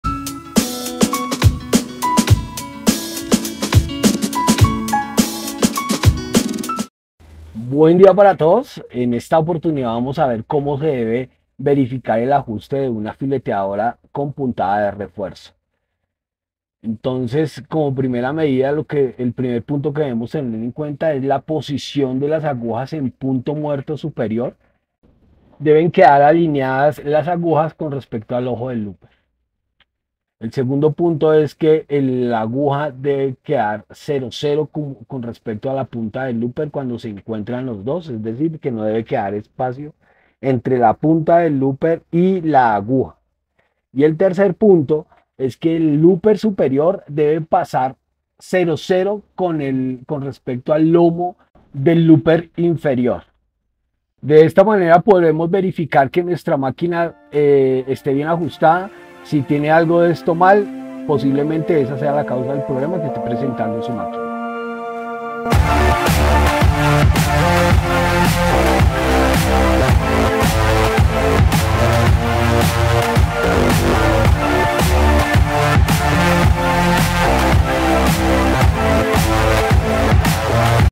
Verificación Rápida de Puntada | Fileteadora
✅Si está presentando alguna falla es posible hacer esta verificación para saber que puede estar pasando.